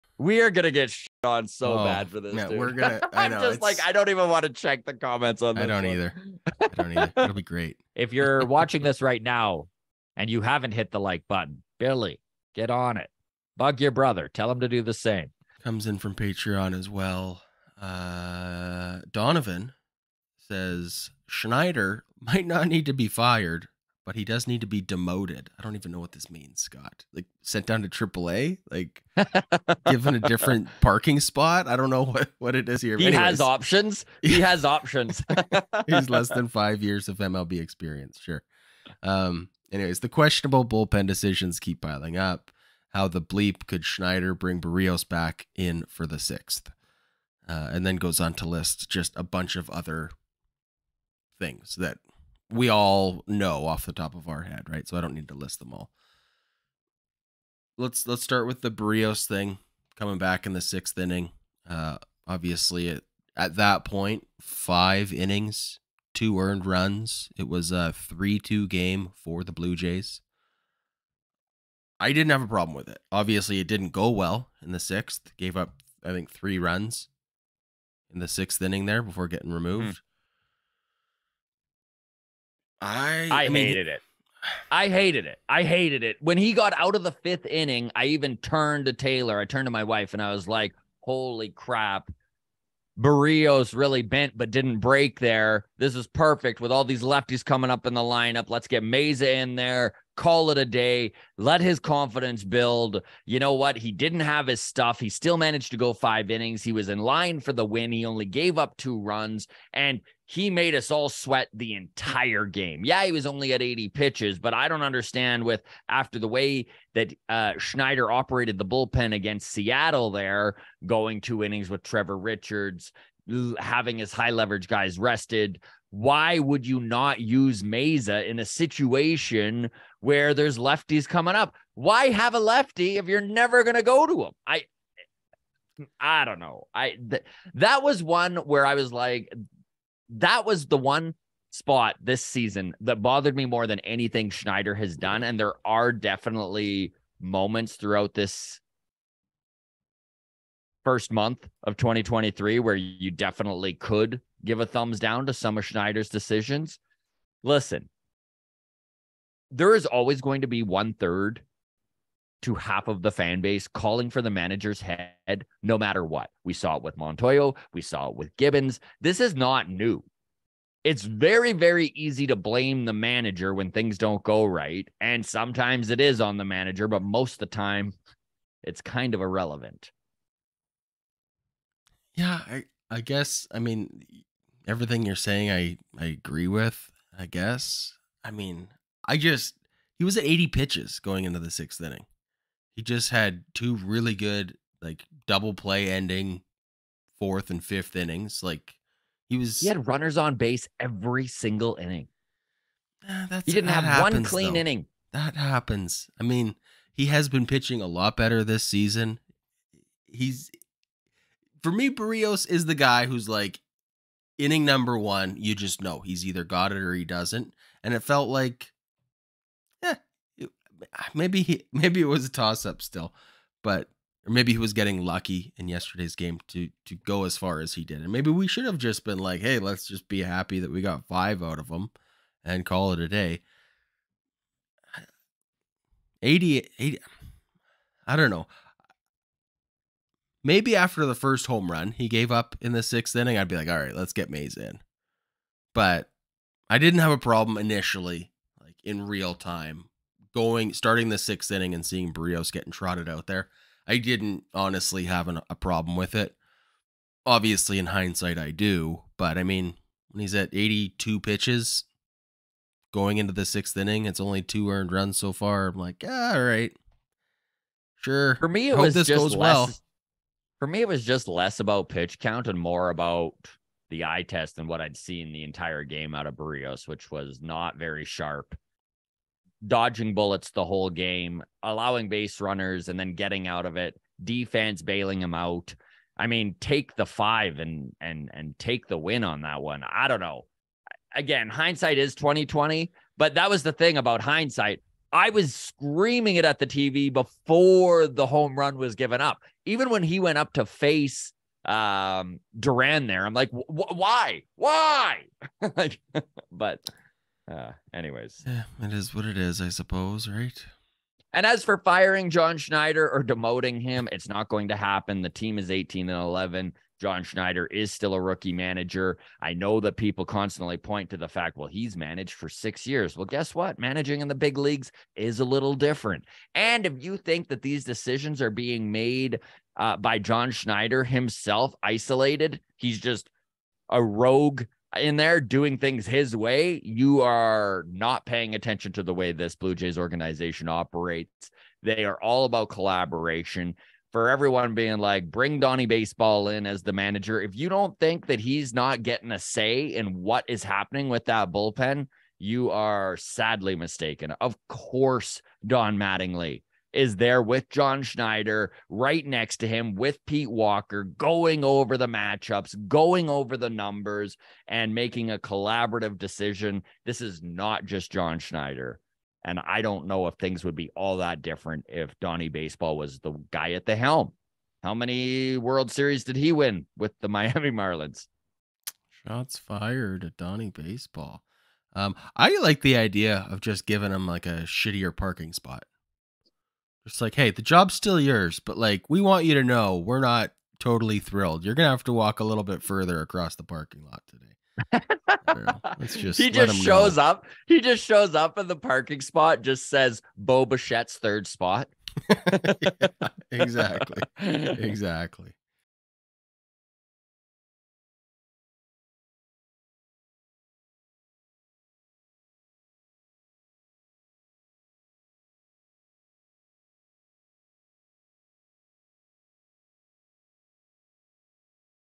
NO, John Schneider should NOT be Fired! - Blue Jays Mailbag - May 2, 2023
Some Blue Jays fans are calling for John Schneider's termination, lets slow down on that!